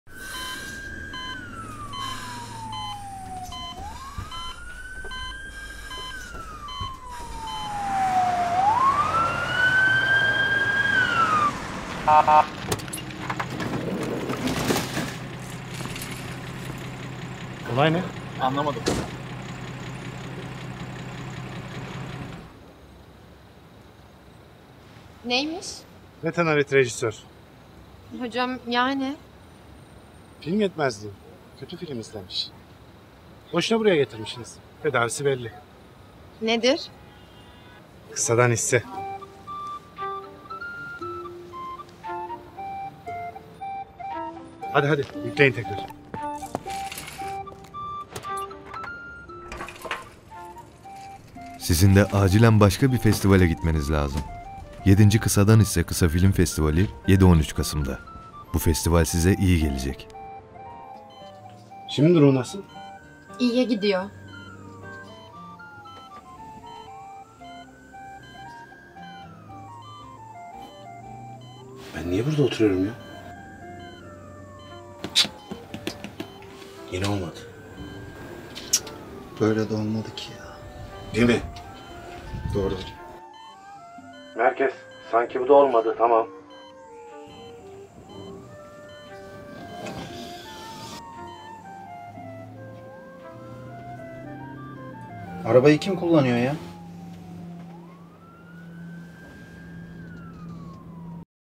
7KHKFF - Festival Tanıtım Filmi / Retanarit Rejisör
Başka Bi' Festival!
7. Kısa’dan Hisse Kısa Film Festivali başlıyor.
Sizin de acilen başka bi’ festivale ihtiyacınız var.
Bu festival size iyi gelecek.

Festival Haftası: 7-13 Kasım.

Kısadan Hisse
Kısa Film Festivali